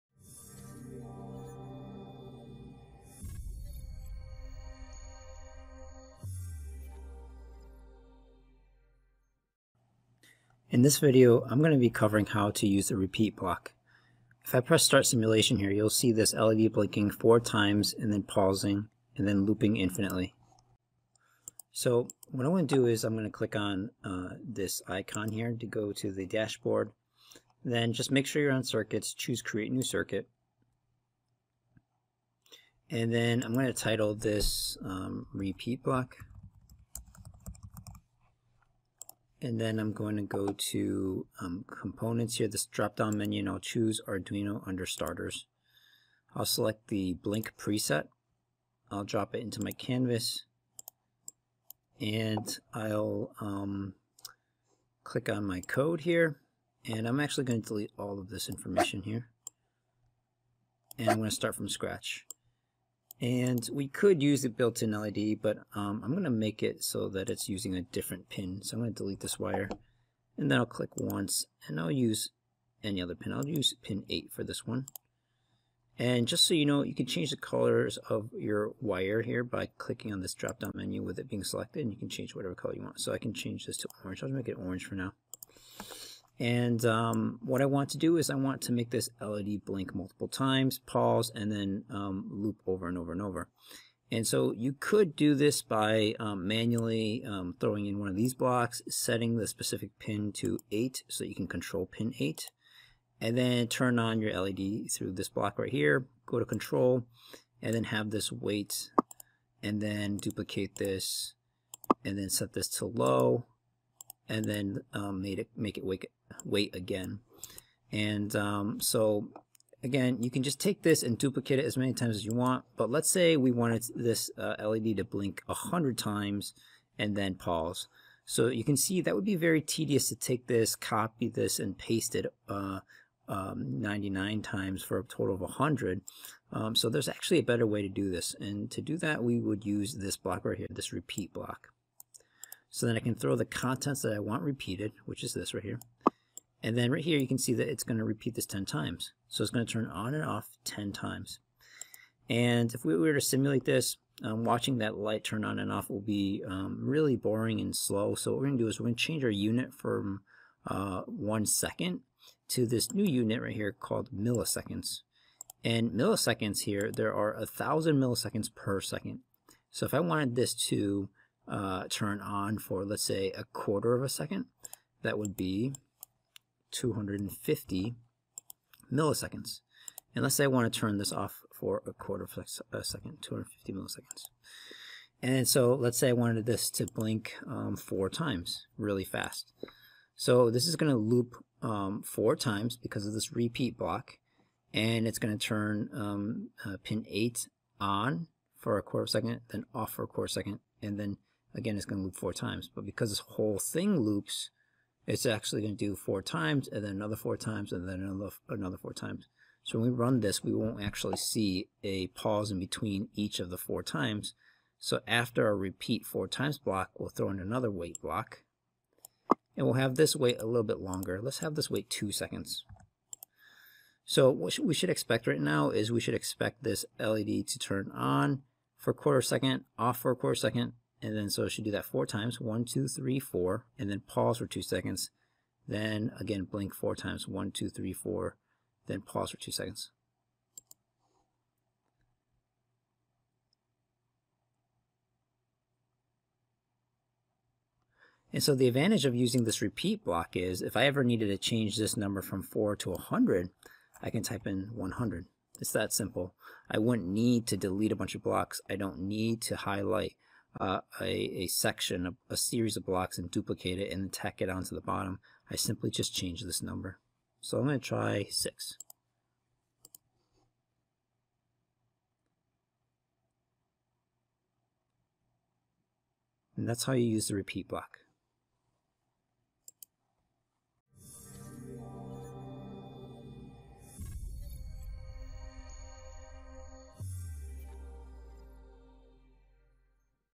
3 Repeat Block
NY Master Teacher Thuc Phan teaches you and your students how to use TinkerCAD, Arduinos, and block coding to make an LED blink using the repeat command. for more Arduino projects under "control systems".